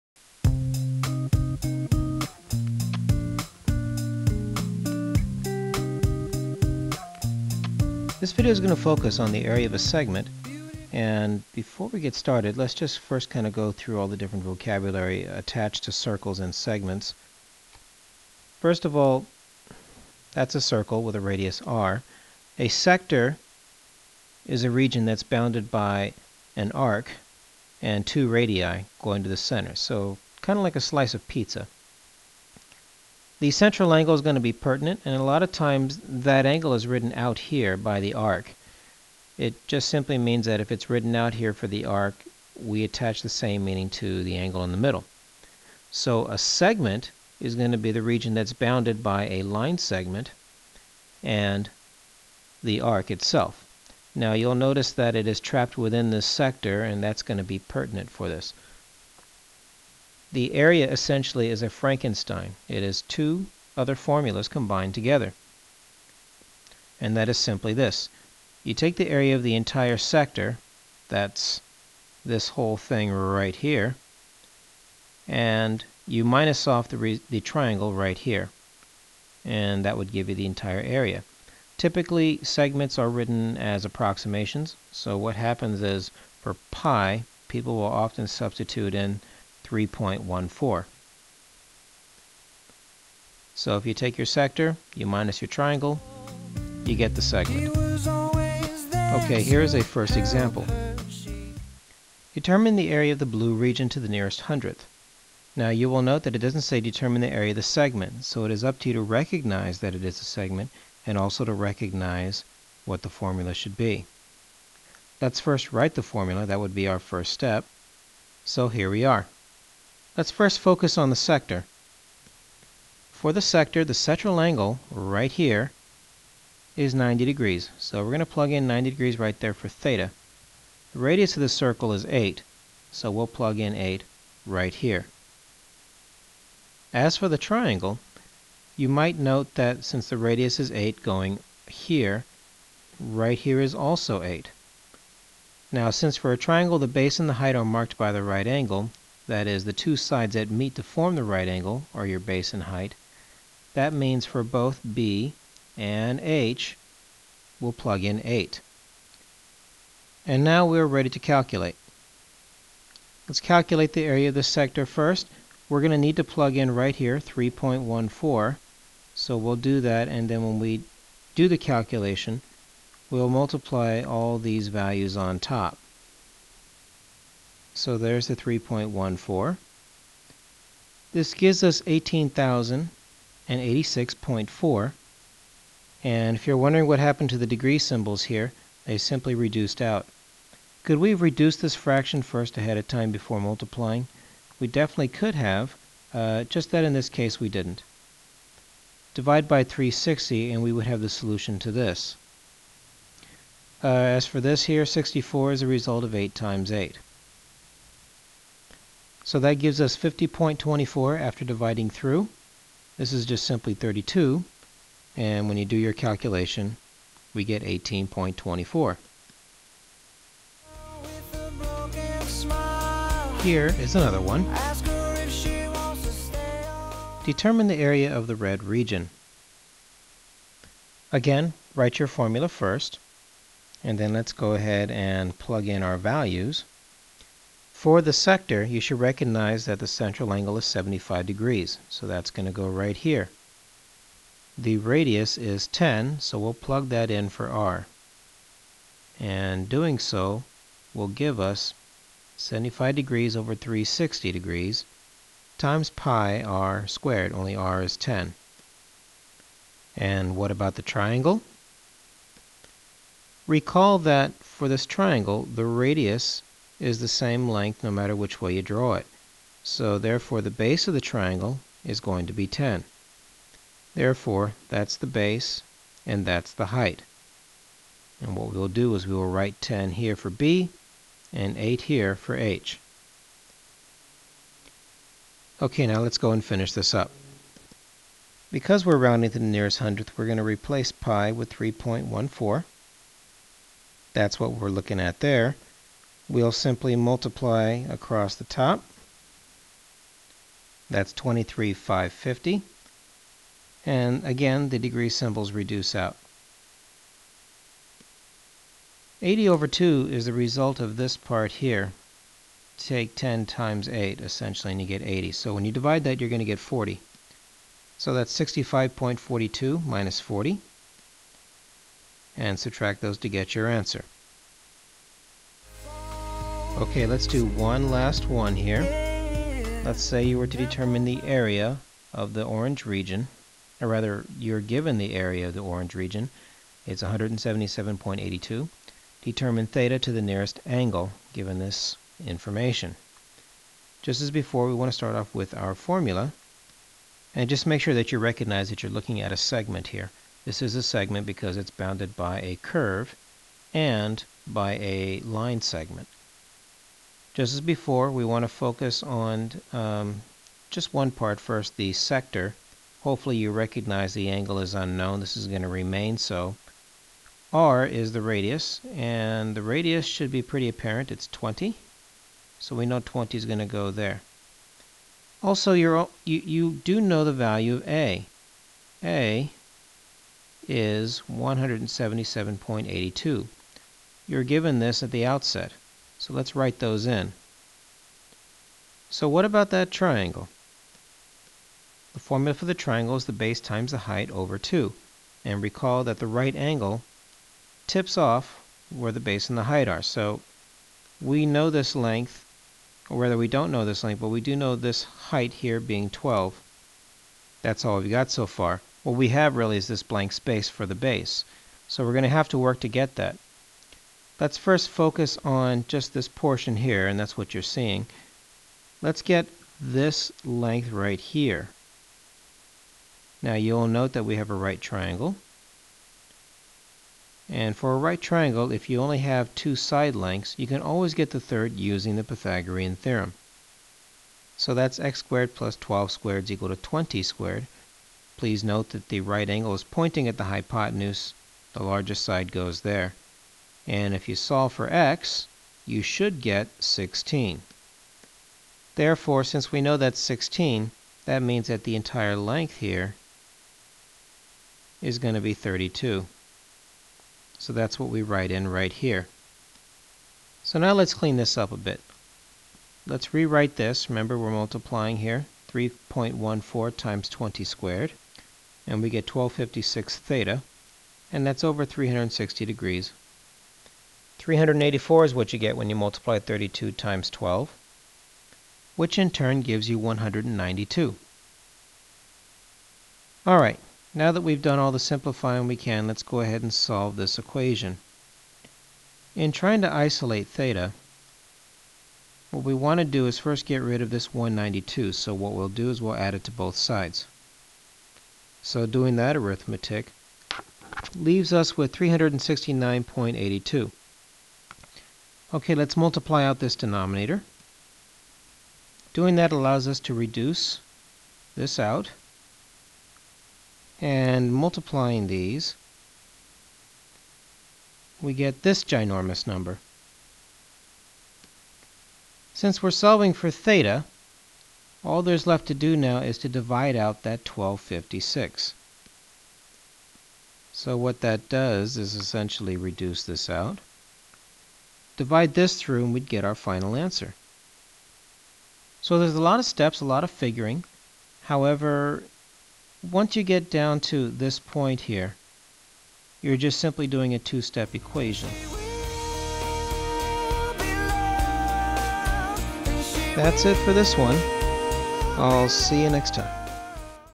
Geometry - Area of Segments
This lesson shows three examples of how to calculate the area of segments of circles.  Segments are regions bounded by an arc of a circle and a chord (line segment).  Introduction to area of  sectors and triangles are prerequisite to this video.